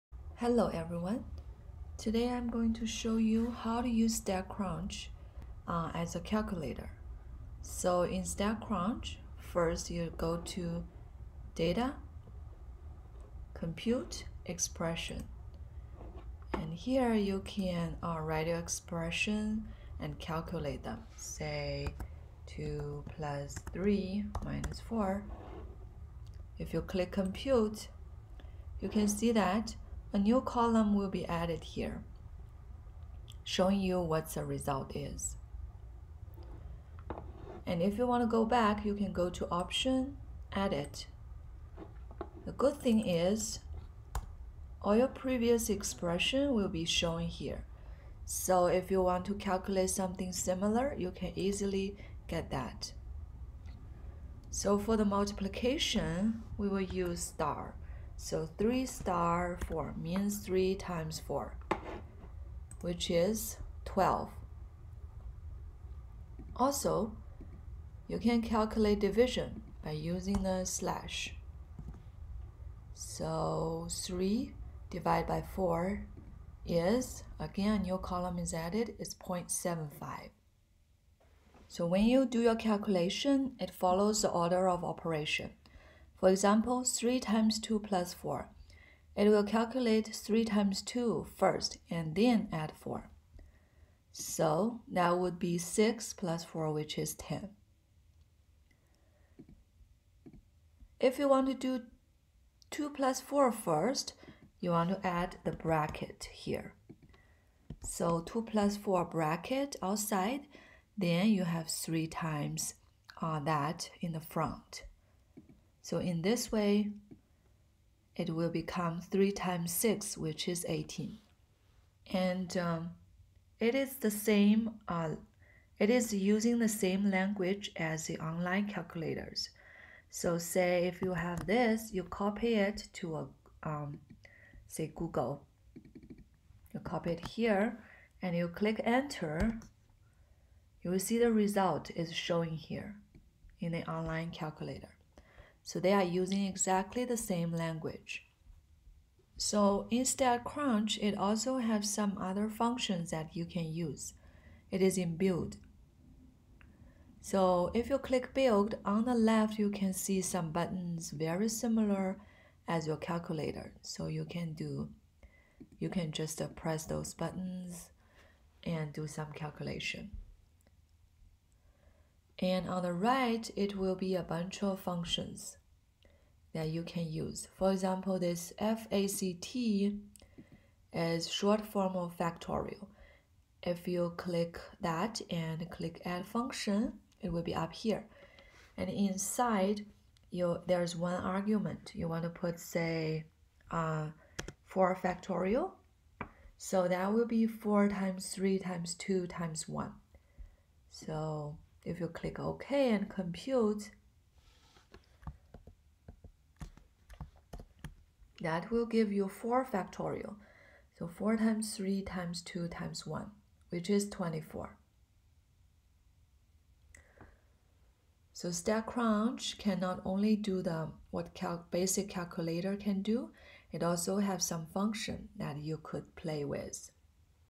How to Use StatCrunch as a Calculator
In StatCrunch, you click Data, Compute and then Expression. You could use StatCrunch as a calculator here.